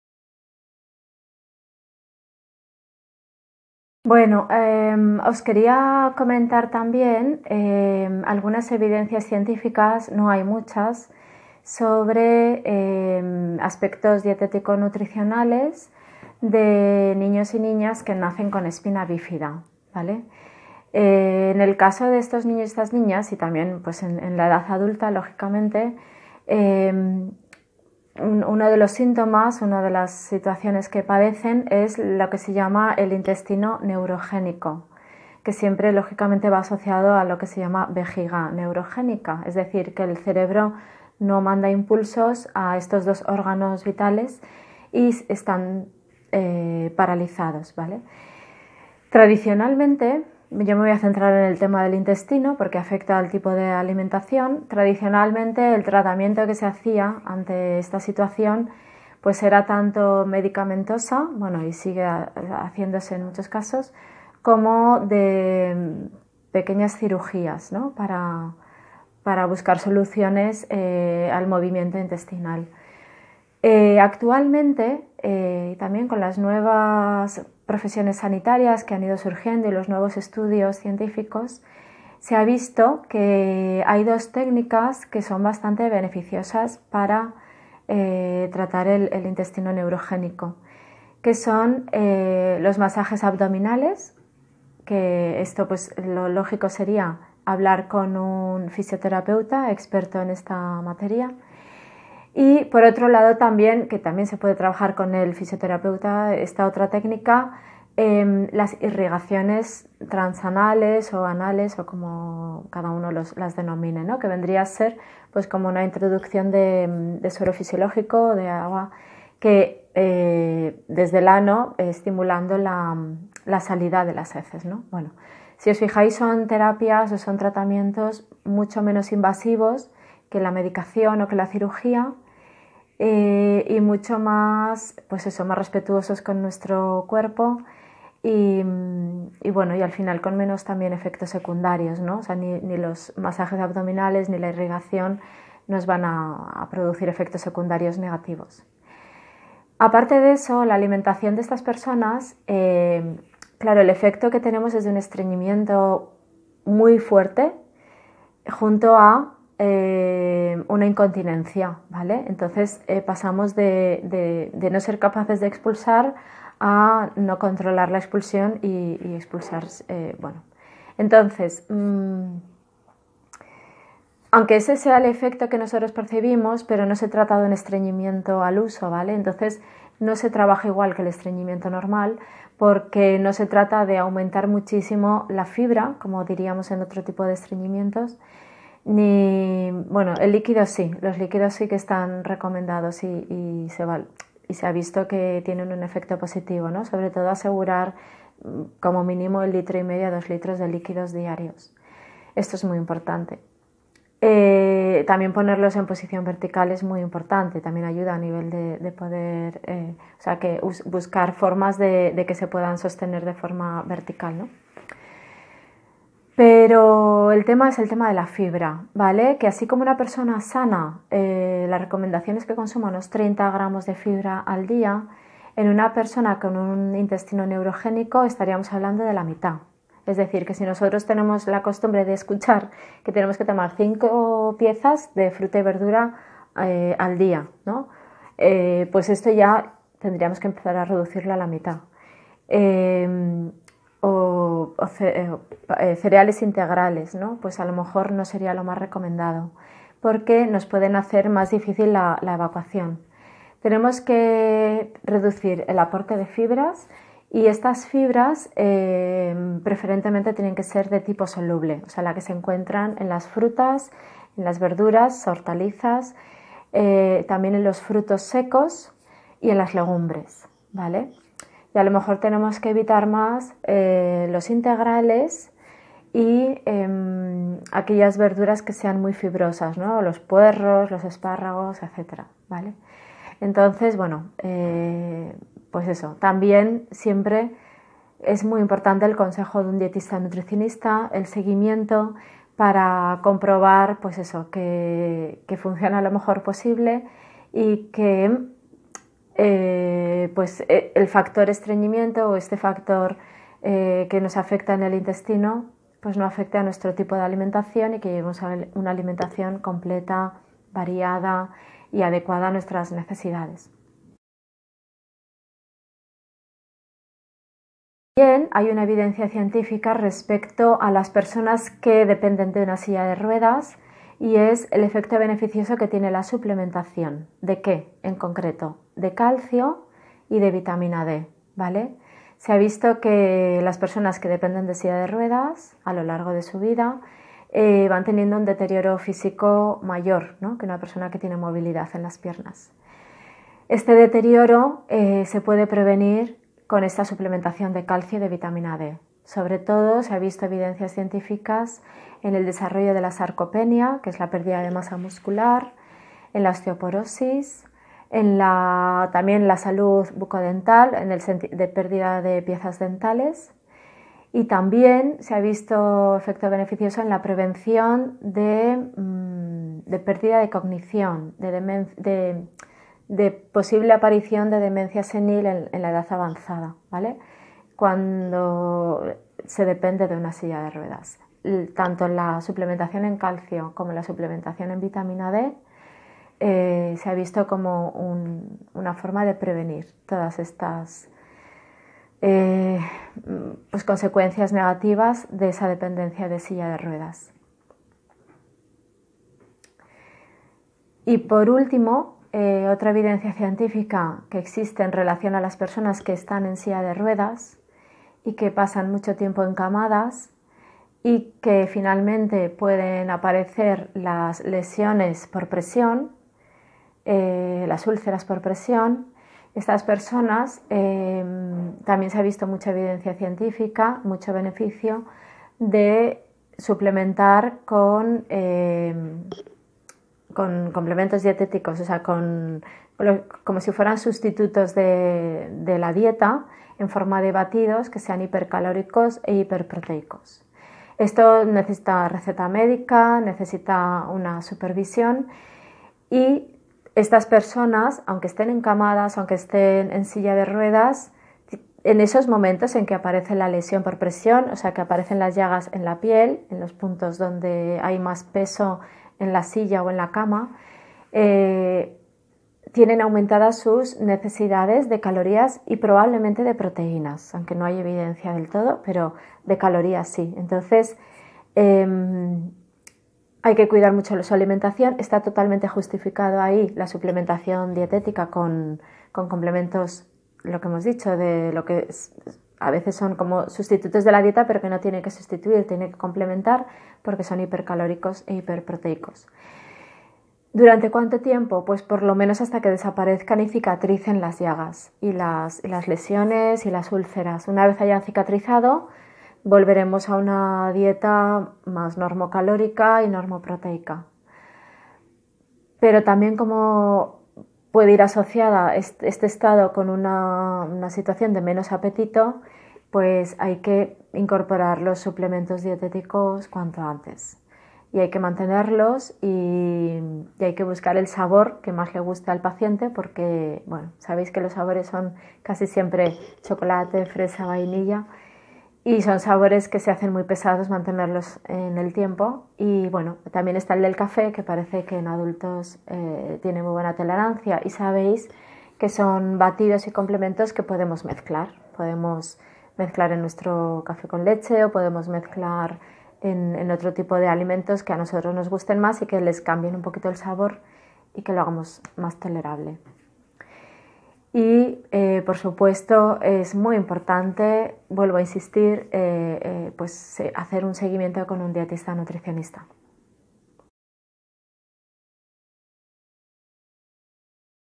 Espina bifida y personas en Silla de Ruedas: evidencias en nutricion
No existen muchas evidencias cientificas en nutricion de personas con espina bifida, intestino neurogenico o en silla de ruedas, pero las que existen te las cuento aquí.